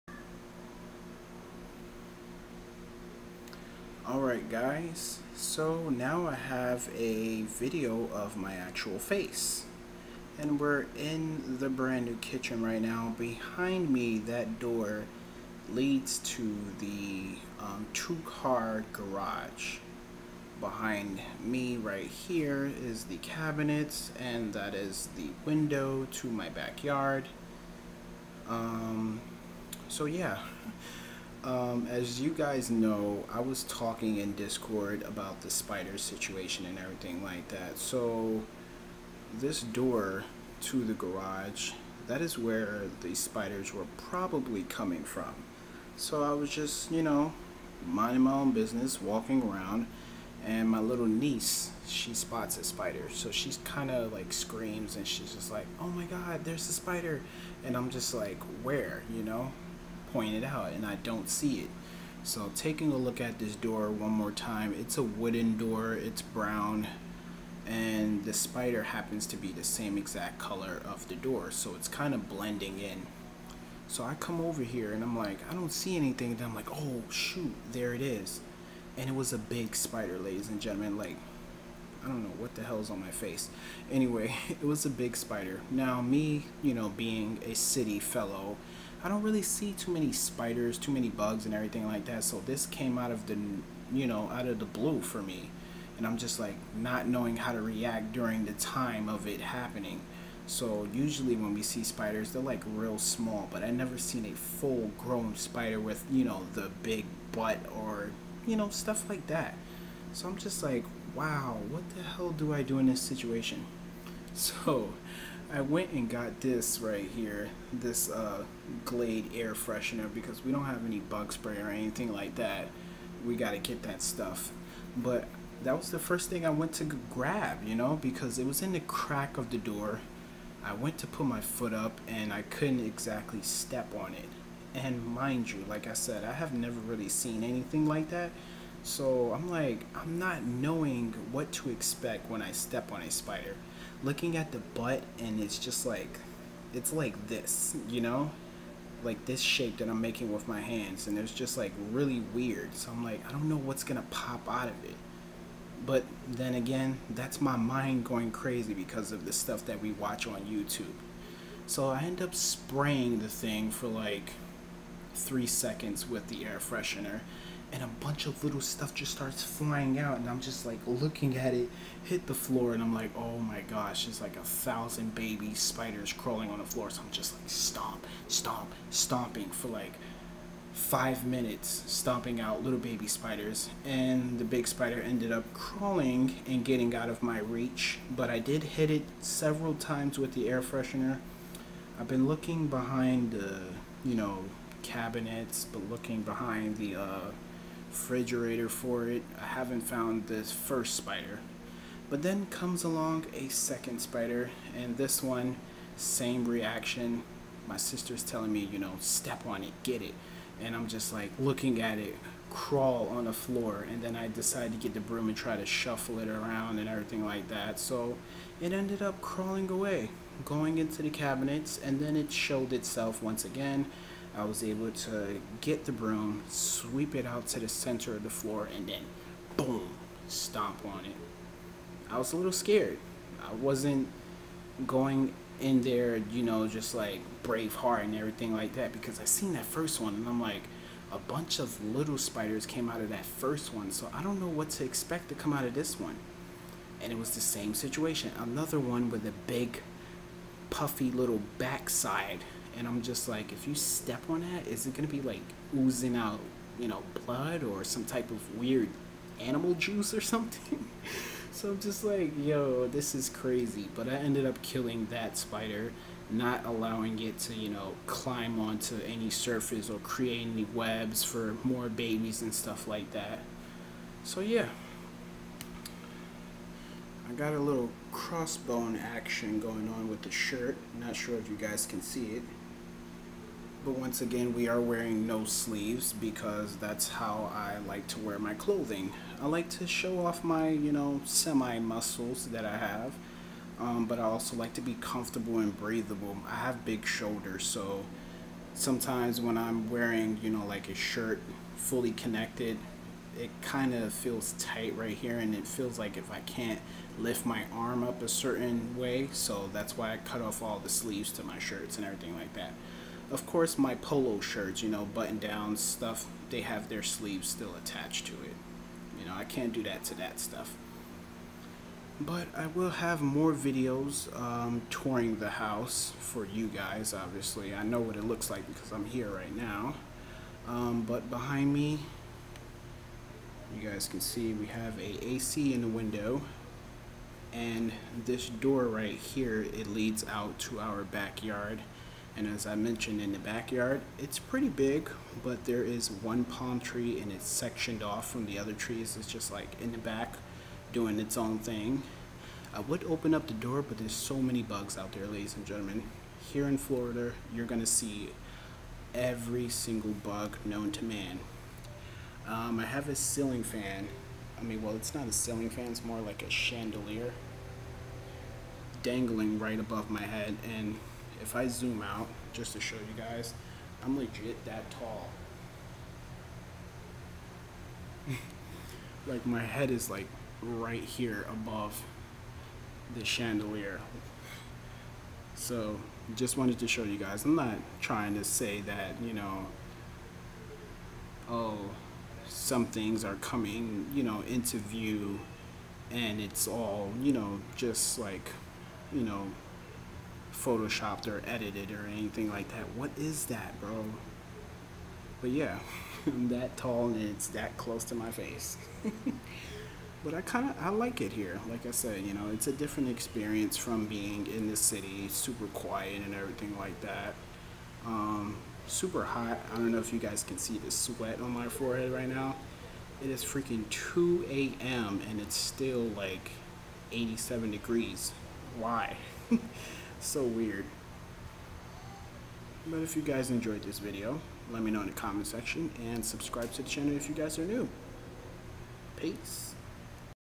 Spider Story (Funny-ish Must Listen)
Never seen actual alive big butt spiders.
I'm a noob for that LOL I guess
They are scary things to think about and to see